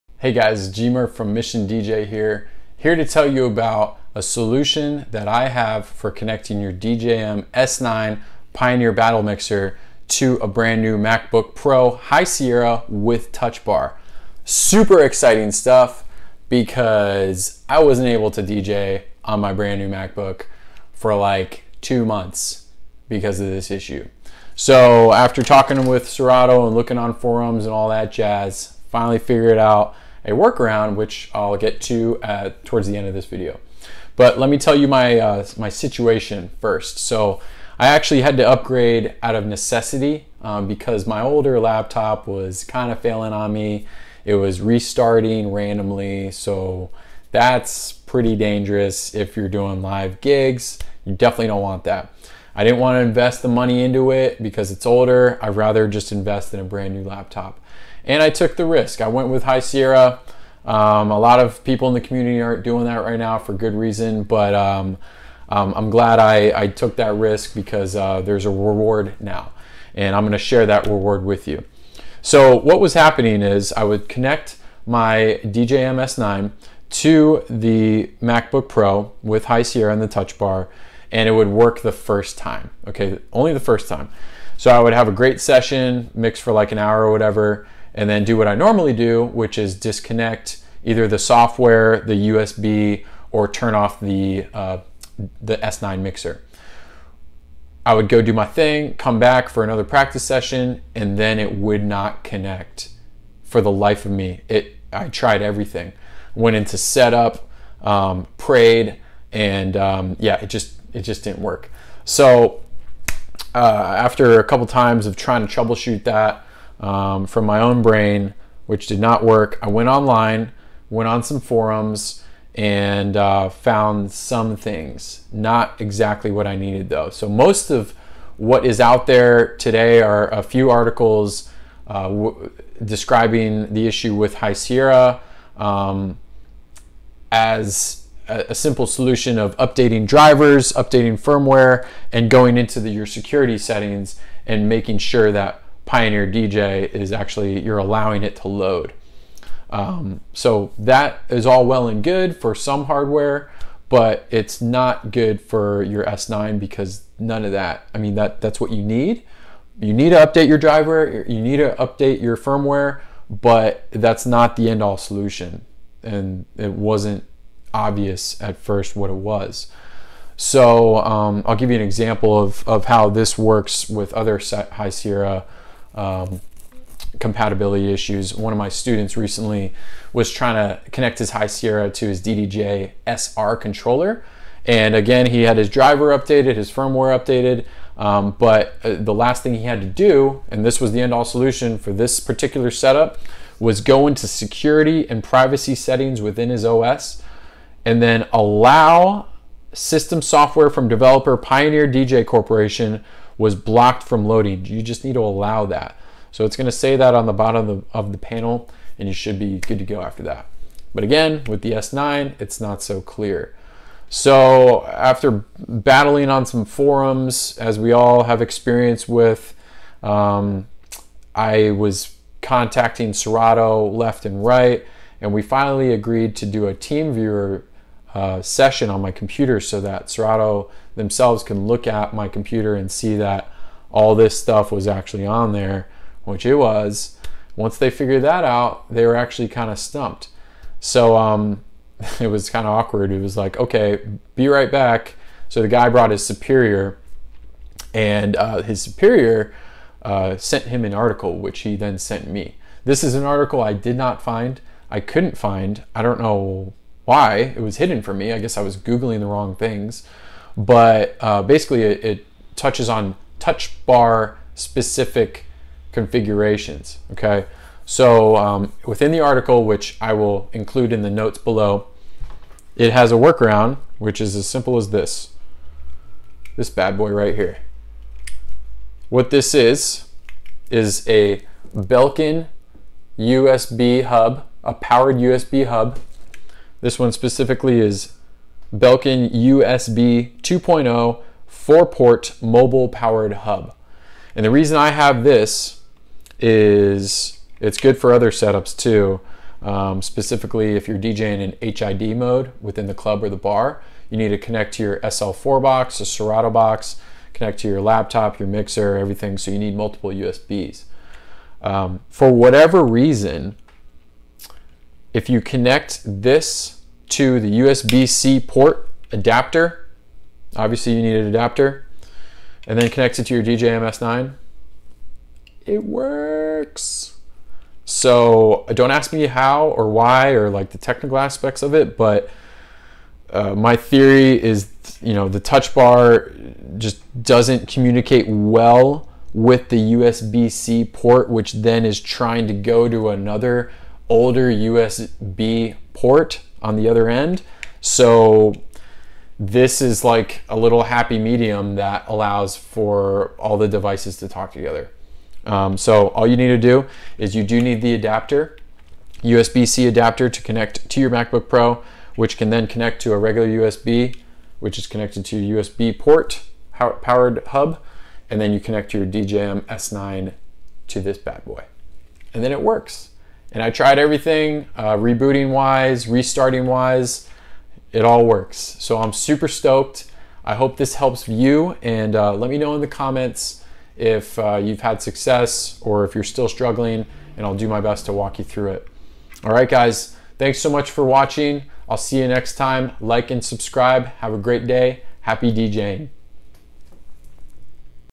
DJM-S9 High Sierra w/ Touch Bar Workaround
Explanation of issue behind connecting Your DJM-S9 to High Sierra w/ Touch Bar MacBook Pro and workaround.